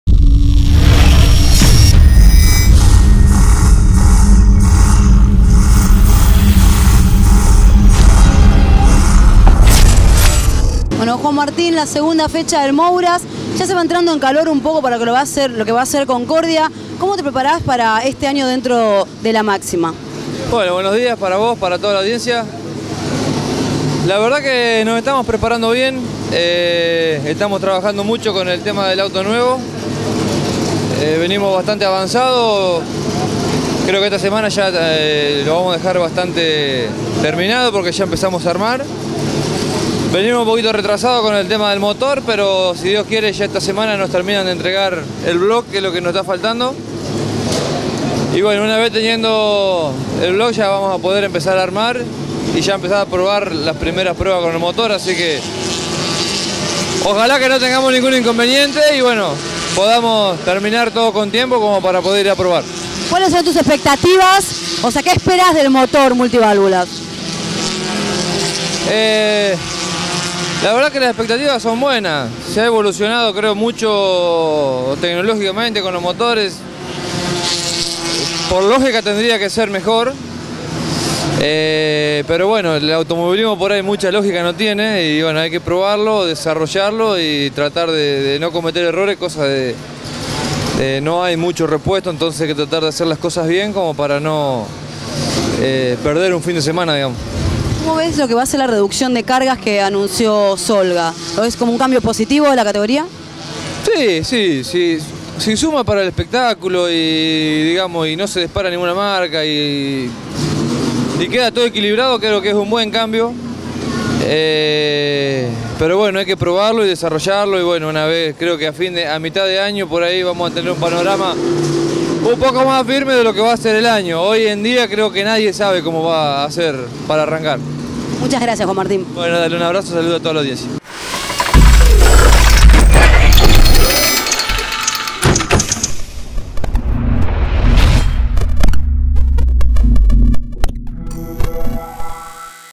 Toda la previa del Turismo Carretera 2015
Juan Martín Trucco, nos cuenta de que forma se alista para disputar la Temporada 2015 del Turismo Carretera.